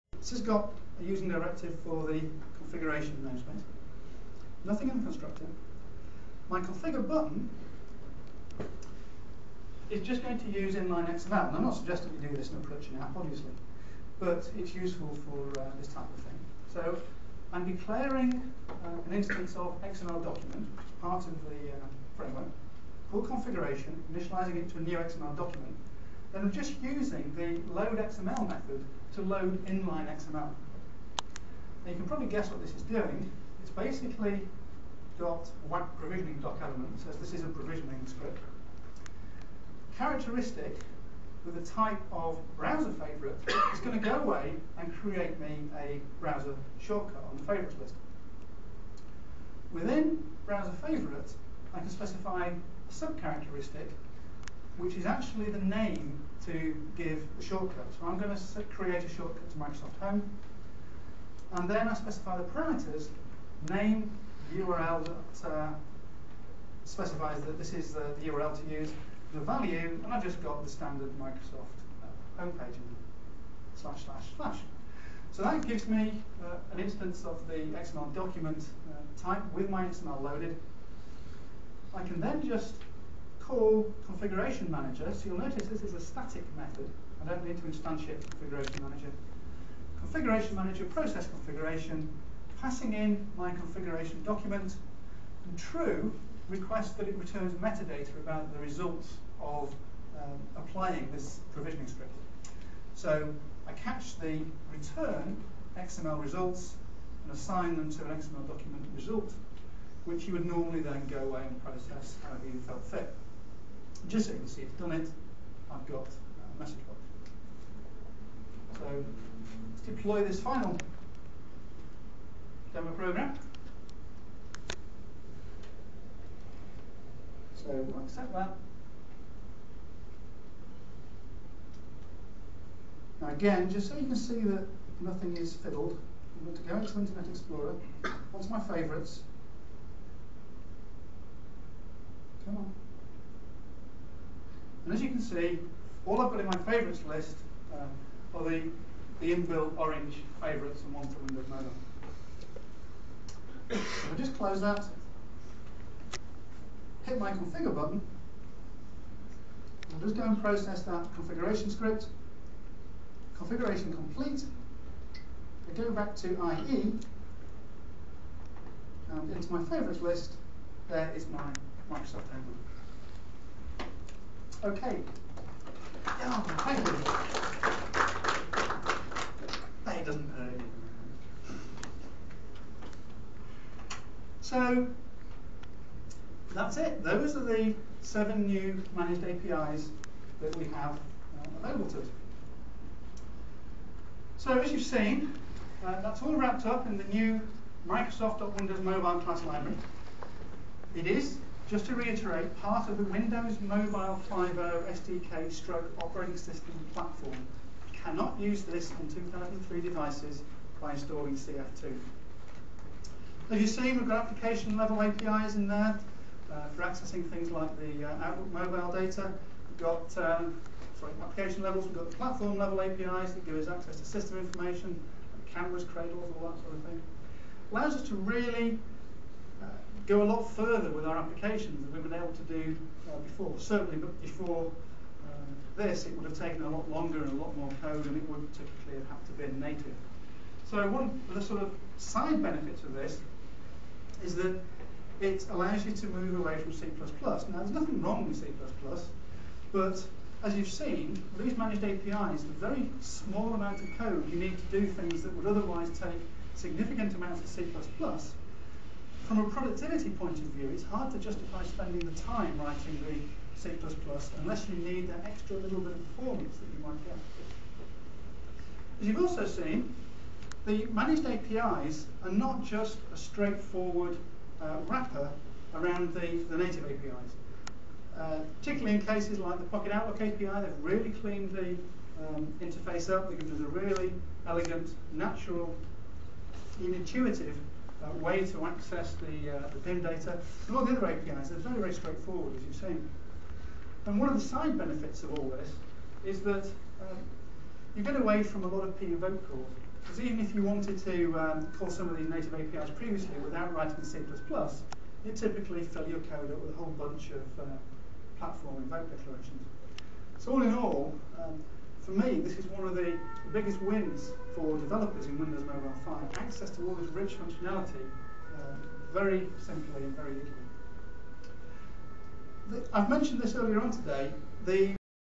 MEDC: Managed Code on Windows Mobile 5.0
This is part of a session about developing managed (.NET 2.0 CF) code on Windows Mobile 5.0 devices and Visual Studio 2005.